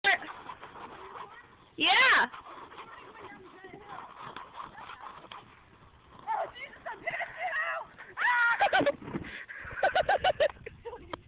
The Sledding Adventure - Courtney - Take #1
Courtney tries to sled and fails miserably.